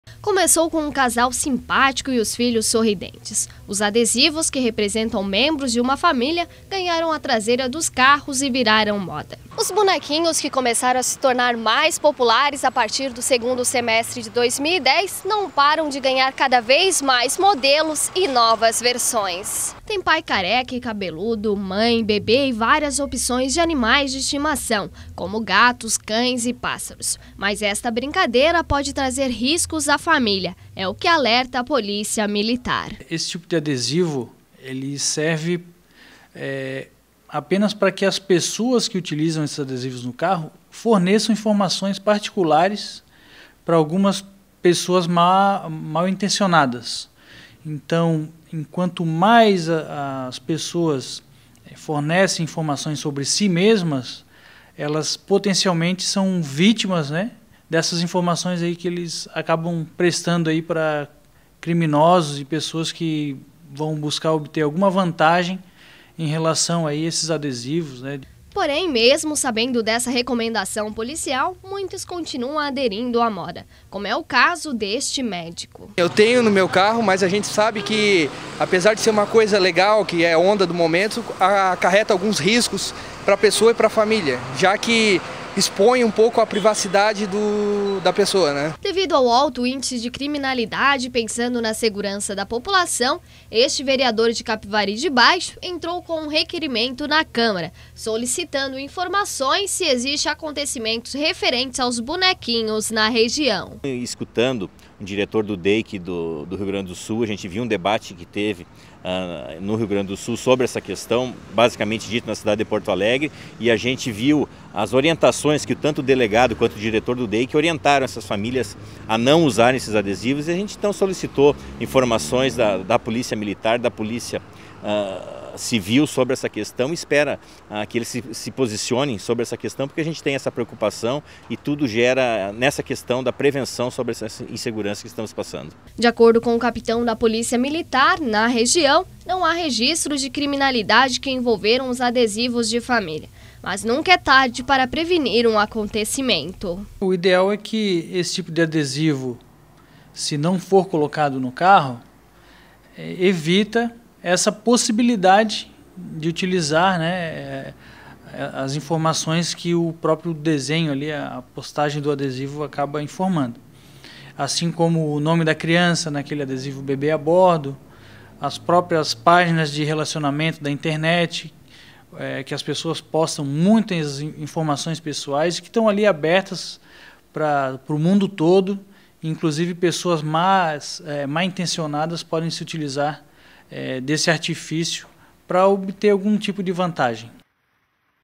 Adesivos de Família | Perigo | Unisul Tv | Tubarão - SC | 2011
É só andar nas ruas e parar um pouco para perceber nos carros a quantidade de adesivos da família. A febre é na verdade uma grande brincadeira e virou diversão principalmente para as crianças. Mas existe também um outro lado que preocupa as autoridades. Com o aumento da criminalidade muitos bandidos podem usar as informações desses adesivos como informação para atos de violência.